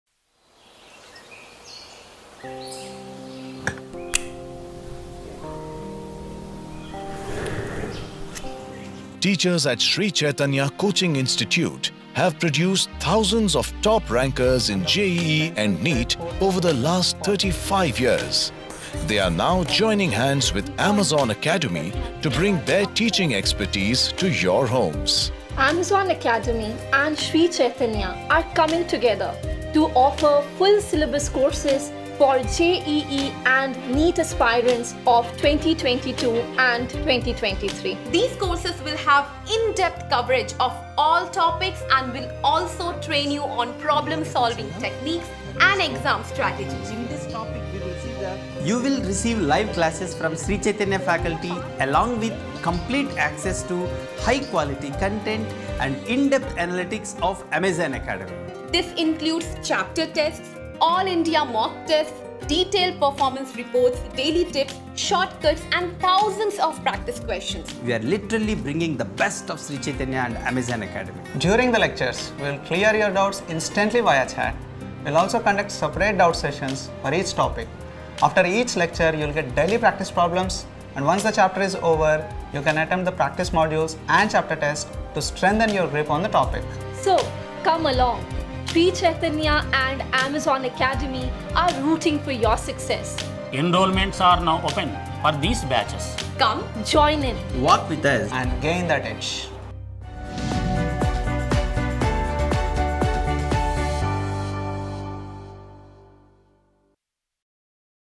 Prepare for JEE/NEET with Sri Chaitanya’s expert faculty on Amazon Academy
Enrolments are now open for Sri Chaitanya JEE/NEET Repeater Batches.
This course gives you access to 400+ hours of live lectures taken by expert teachers from Sri Chaitanya Institute, Structured study plan including daily practice problems and class notes after each session, Dedicated teachers to clear doubts during Live Classes, Regular and dedicated doubt clearance classes, Thousands of questions for practice and self-assessment.